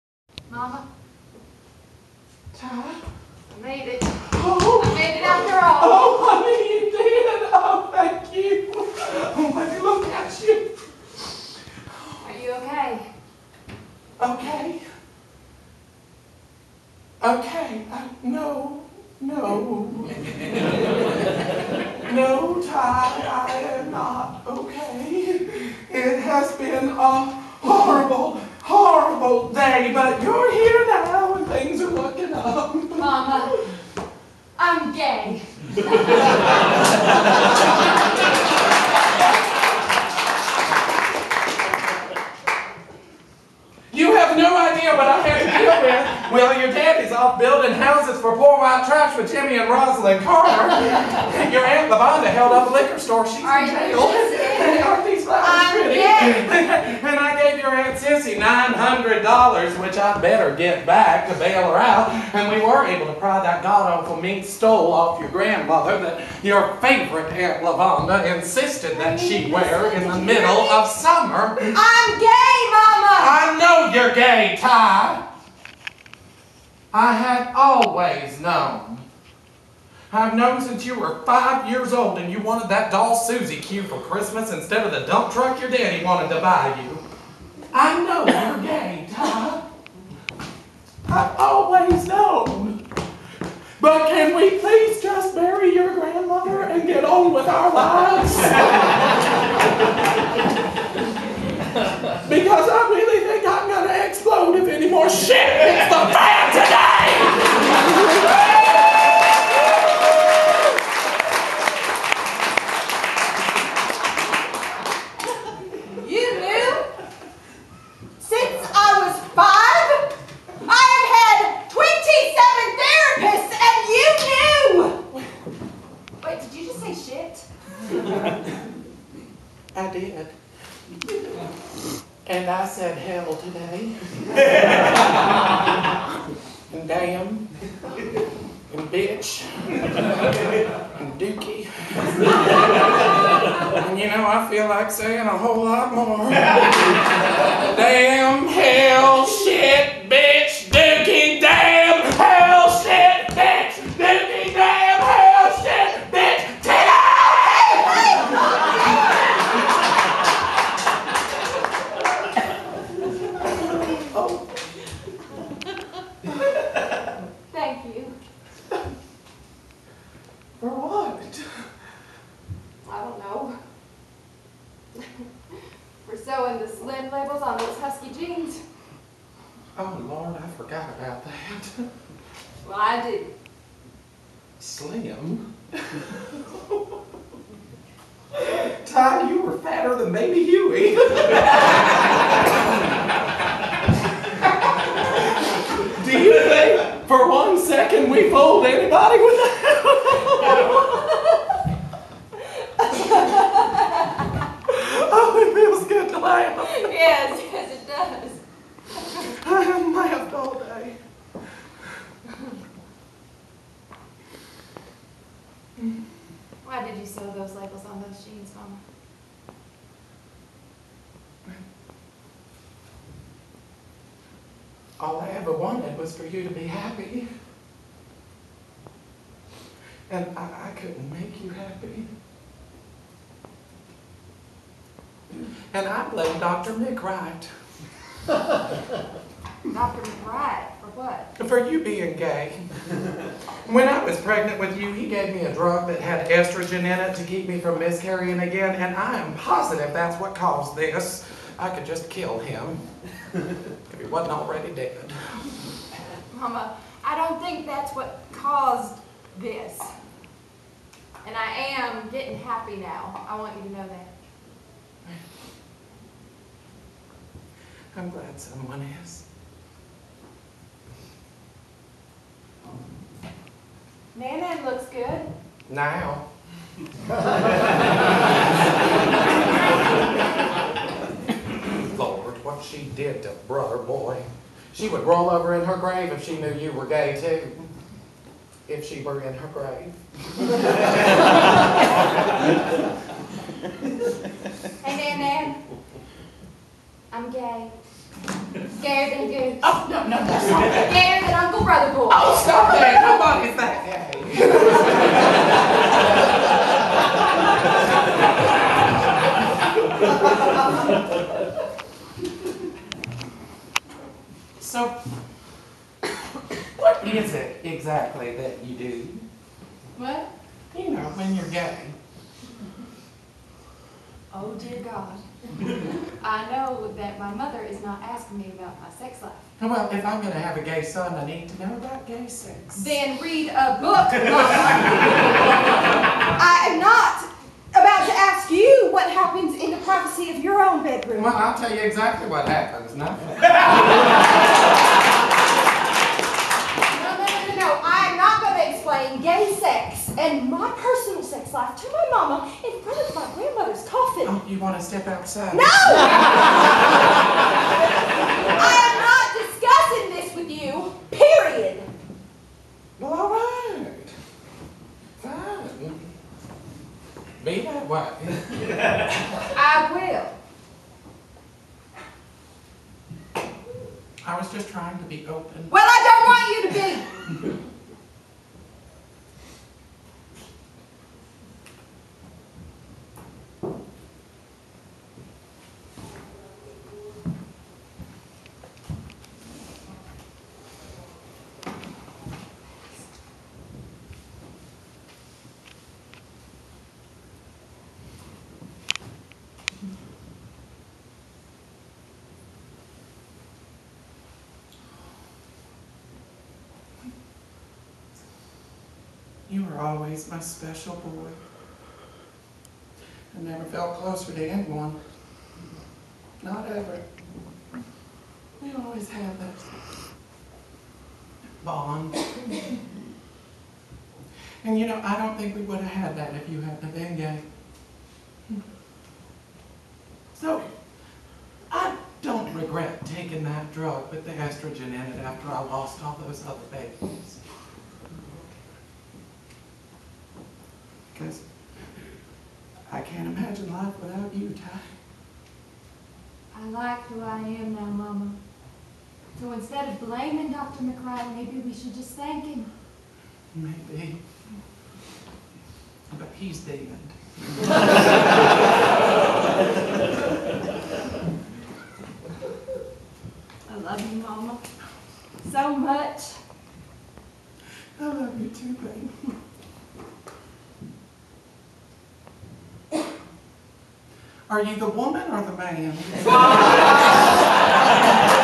"Sordid Lives: It's a Drag" ~ Ty Comes Home.
From Process Theatre Company's 2009 cross-gender staged reading of Del Shores' "Sordid Lives," directed by DeWayne Morgan.
  Ty Williamson (Barbara Cole-Uterhardt) returns to Texas for his grandmother's funeral, and has a heart-to-heart with his mother, Latrelle (Topher Payne).